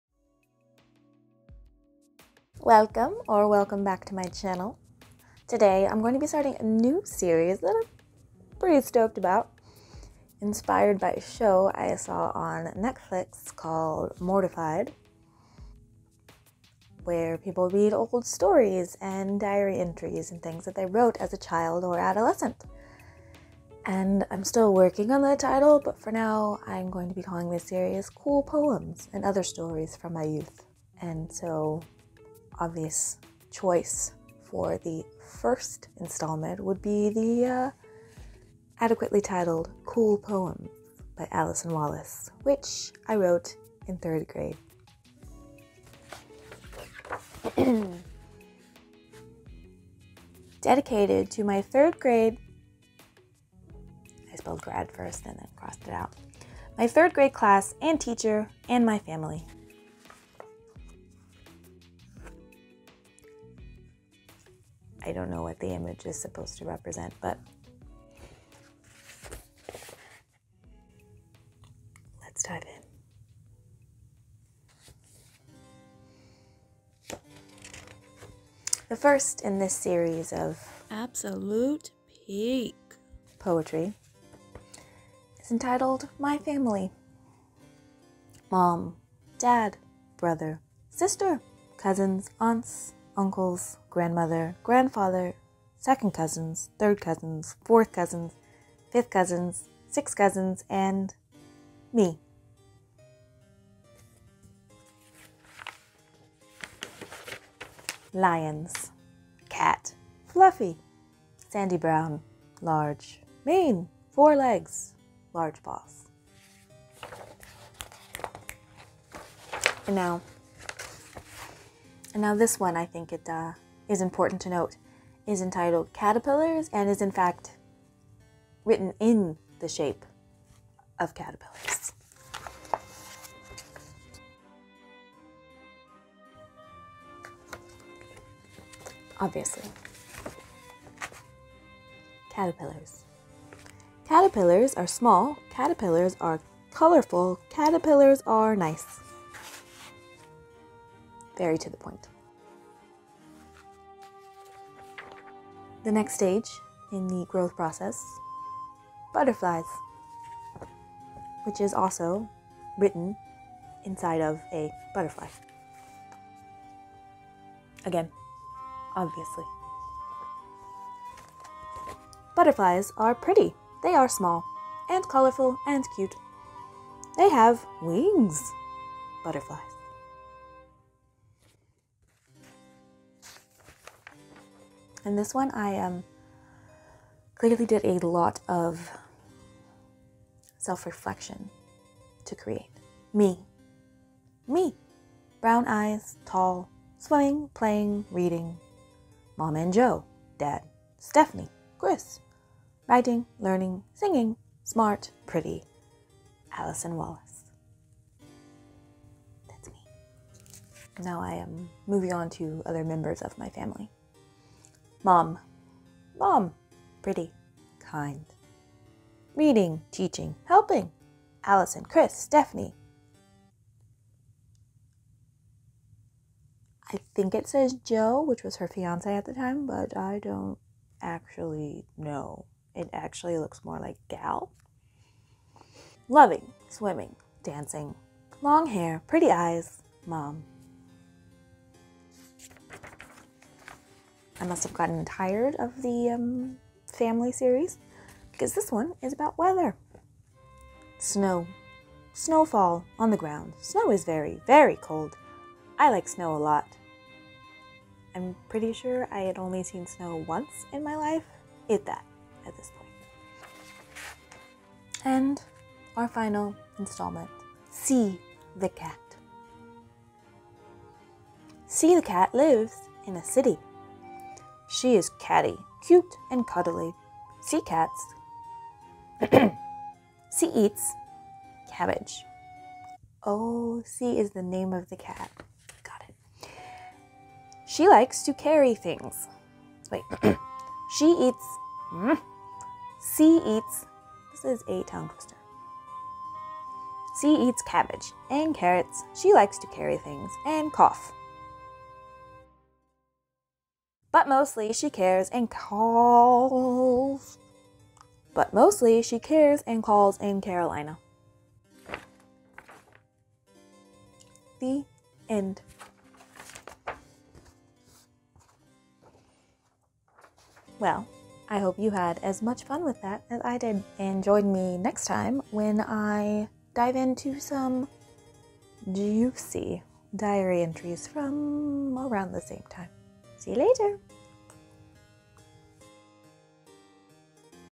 Butterflies Have Wings🦋
Since I moved back to Texas recently, I've been sorting through the few boxes I had stored here of things from my childhood and University years. I have found some absolute GOLD STAR items. And, since story time with my old diaries was already a beloved pastime of mine and some of my besties, I thought, why not share the love? Everyone deserves belly laughs.

This is the first in a recurring series, so no worries if 9 year old poetry isn't for you. I was a very dramatic child, with a lot of feelings(who'd have guessed that?), so I bet there will be something for you to laugh at along the way-even if it is just laughing at the often cringeworthy and ever relatable experience that is growing up.